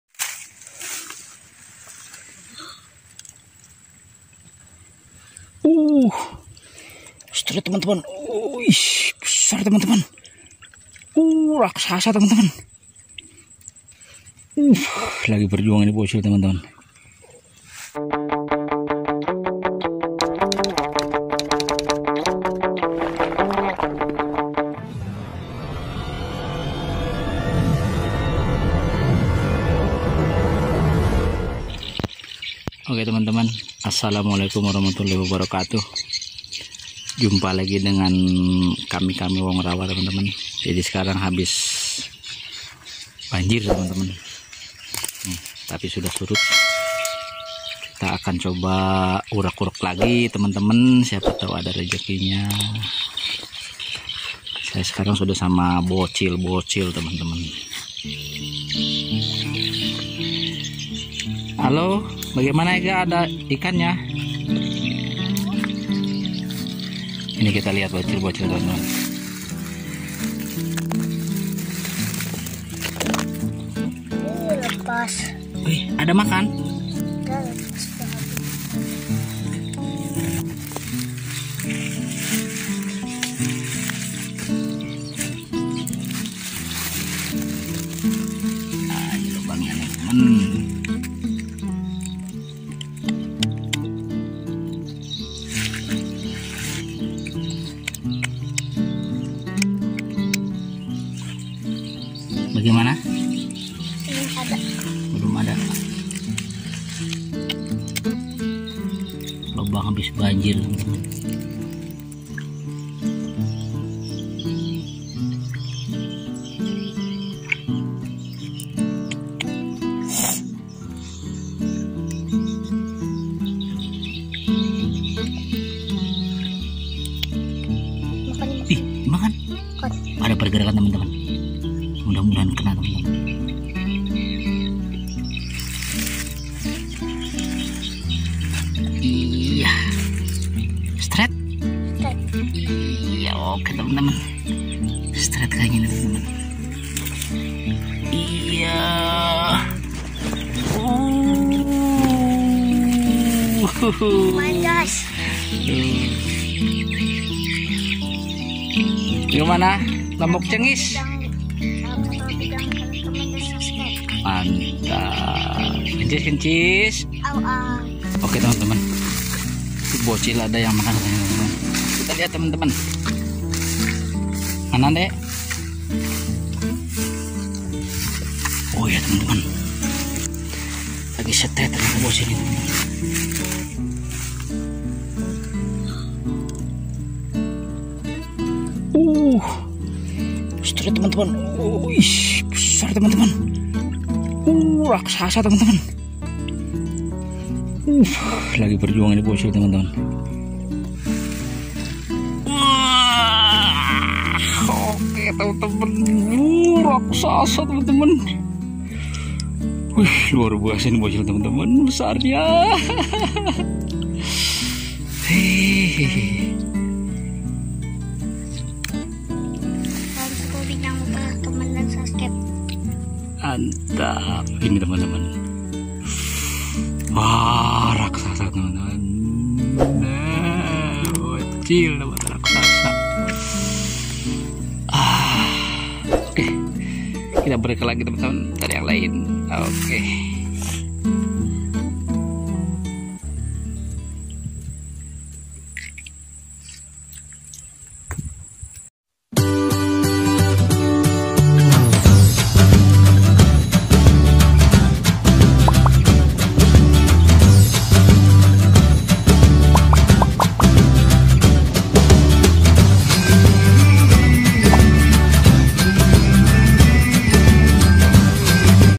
Tes mancing belut setelah banjir || mancing belut lapar
Alhamdulillah kali ini bocil kembali mancing lagi